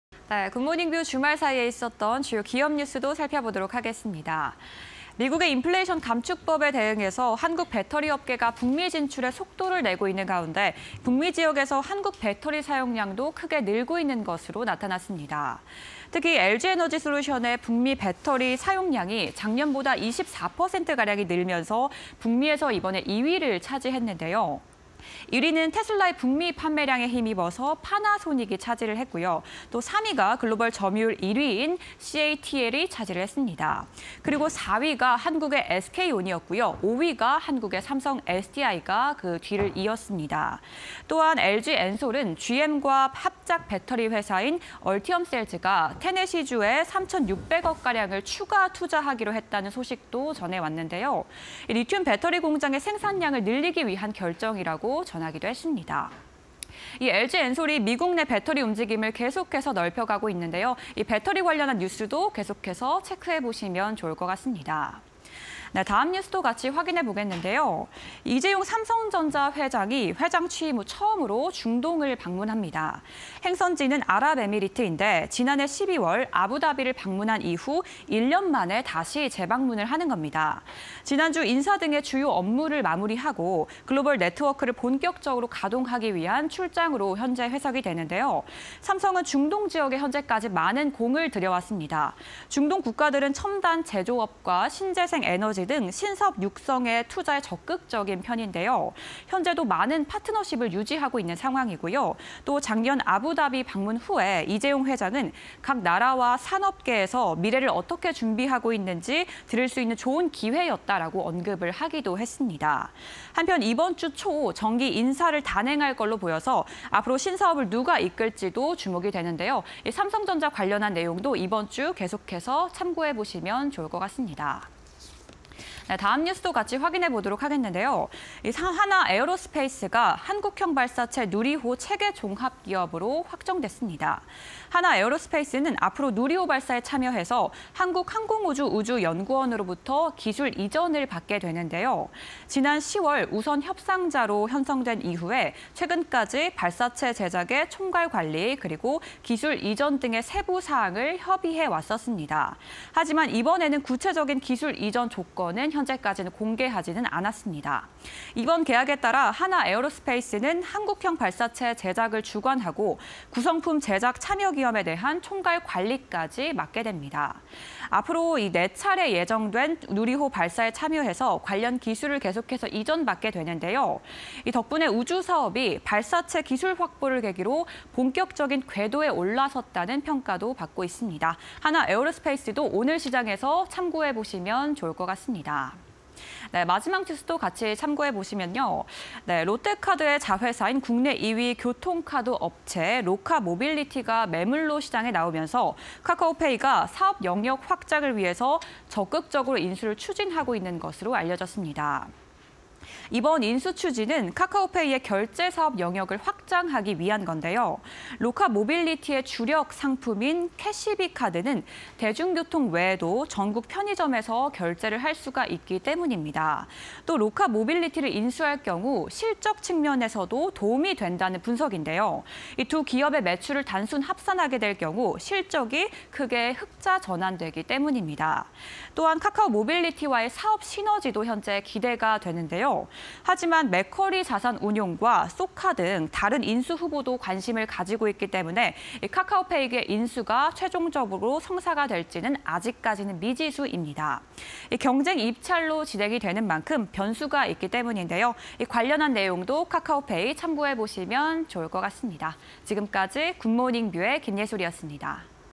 LG에너지솔루션, 삼성전자, 한화에어로스페이스, 카카오페이_공시&뉴스 (20221205)
- 북미 배터리 점유율 2위
- 북미 배터리 사용량 작년보다 24%↑
- 4위 SK온, 5위 삼성SDI
- 얼티엄셀즈, 테네시주 약 3천600억 추가 투자

- 이재용 회장, 취임 후 첫 중동행
- 지난해 12월 이후 1년만에 UAE 재방문
- 중동 국가, 신사업 육성 위한 투자에 적극
- 이번주 초 정기 인사 단행… 신사업 주목

- 한우연과 ‘누리호고도화사업’ 계약 체결
- 2027년까지 ‘누리호’ 4차례 추가 발사 계획
- '발사-위성서비스의' 밸류체인 구축
- “추가 발사 성공해 한국의 우주산업 발전 기여”

- 교통카드업체 ‘로카모빌리티’ 매물
- 카카오페이 적극 인수 추진
- 결제사업 영역 확장 및 실적 개선 효과
- 맥쿼리자산운용, 쏘카 등 입찰 참여